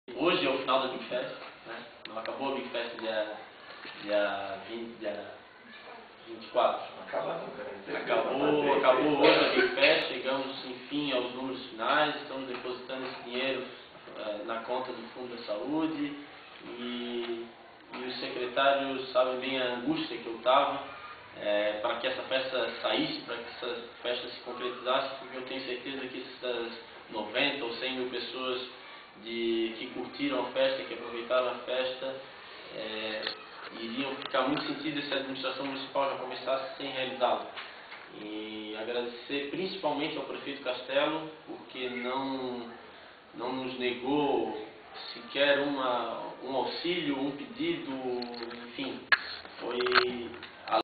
Futuro Hospital de Biguaçu ganha mais de R$ 100 mil da BigFest (05/06)
Do montante arrecadado de R$ 426.325,00 nas bilheterias da 13ª BigFest, R$ 106.581,25 foram repassados para a construção do Hospital. Este é o resultado da promessa feita pela Prefeitura, onde 25% da renda será revertido em benefício do Município. Foi uma ideia do prefeito Castelo, além de proporcionar diversão ele queria que da festa saísse frutos. Agora teremos mais de R$ 100 mil reais a serem aplicados na construção do Hospital, disse o secretário Secretaria Municipal de Cultura, Esporte, Turismo e Lazer, Douglas Borba.
Douglas Borba divulgou na tarde de ontem (17), em coletiva à imprensa, a prestação de contas da 13ª BigFest, dando também por encerrada oficialmente o evento.
Chegamos aos números finais, estamos depositando este dinheiro na conta do fundo da saúde, concluiu o secretário.

Público
De acordo com a secretaria cerca de 90 mil pessoas passaram pela festa. Douglas declarou ainda que o público pagante foi cerca de 30 mil pessoas.
Realmente tivemos uma grande presença do público na BigFest, só que infelizmente nem todos compram ingresso. Existe demasiada demanda de ingressos cortesia. Enfim o publico pagante acaba não sendo o que a gente vê lá. Este valor de R$ 426.325,00 foram de cerca de 30 mil pagantes. Tudo está devidamente documentado para não gerar dúvidas, acrescentou Borba.

Diferencias
De acordo com o secretário a edição deste ano da BigFest contou com diferenciais. O primeiro deles foi a transparência, onde a Caixa Econômica Federal fez todo o balanço financeiro. Acompanhamos o trabalho da Caixa Econômica e ninguém pode duvidar dos números apresentados, por esta Instituição pública e idônea, disse.
Outro diferencial foi o show nacional gratuito para a comunidade no dia do aniversário do Município, apenas com a entrega de 1k de alimento não perecível. O parque para as crianças foi aberto em duas oportunidades, gratuitamente para as crianças, completou Borba.
Gravação: Jornal Biguaçu em Foco. Data: quarta, 17 de junho de 2009.